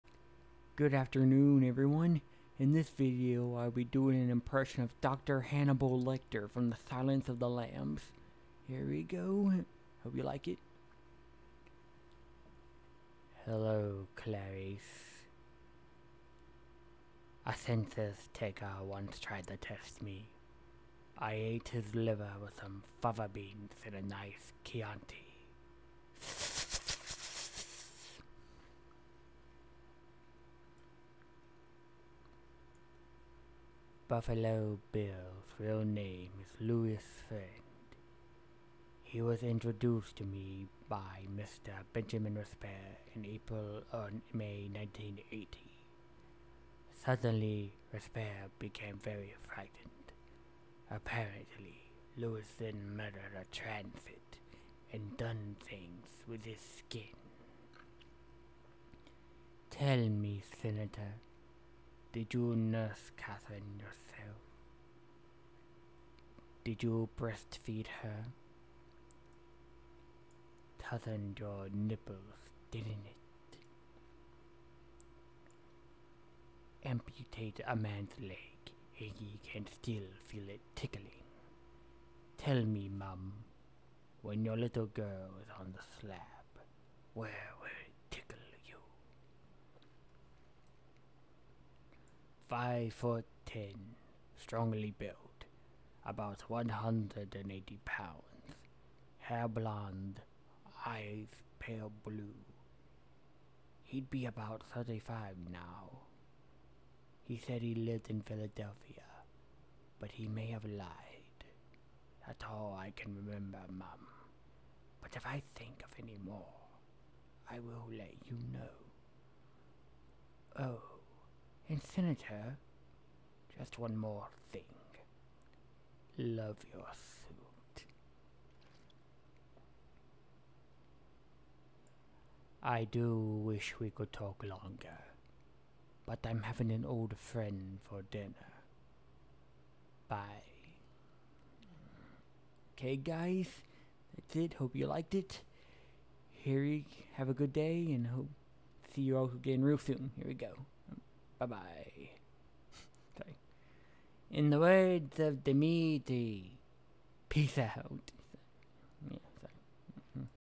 Dr. Hannibal Lecter (The Silence Of The Lambs) Impression
Good afternoon, everyone. In this video, I will be doing an impression of Dr. Hannibal Lecter from The Silence Of The Lambs. Anthony Hopkins delivered a truly horrifying performance to this classic movie villain, and I did my best to deliver that same performance. I hope you all enjoy it and have a good day.